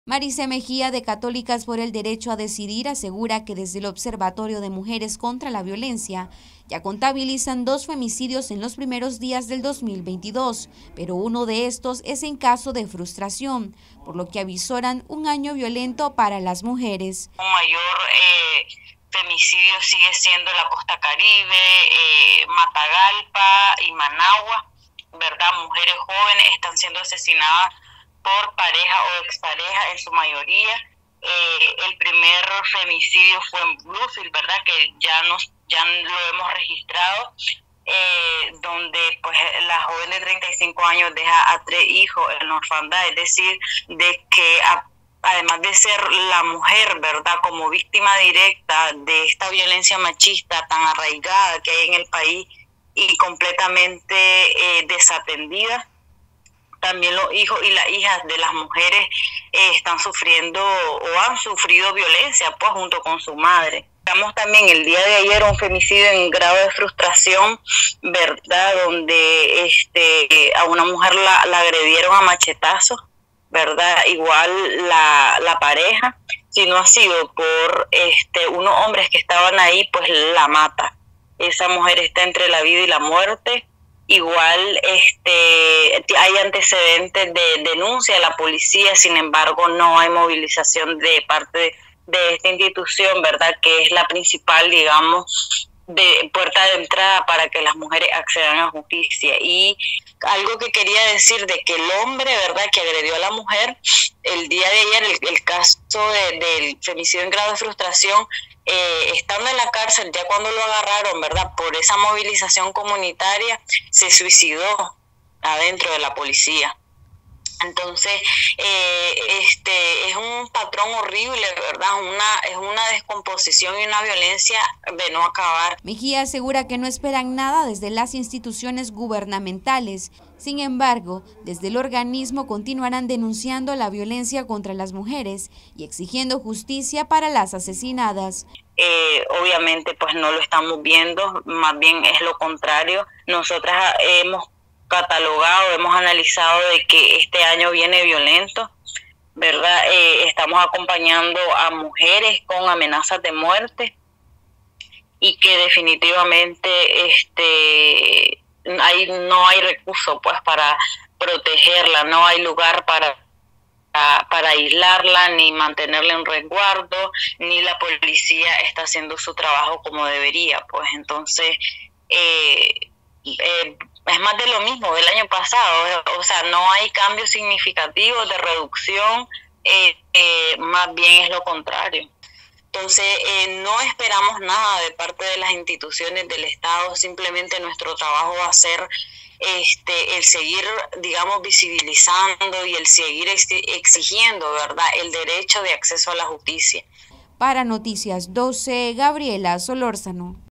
Organización feminista avizora un año violento contra las mujeres en este 2022
Noticias12Nicaragua La organización feminista Católicas por el Derecho a Decidir contabilizan 2 femicidios en los primeros días del 2022, uno de ellos en caso de frustración, por lo que avizoran un año violento para las mujeres.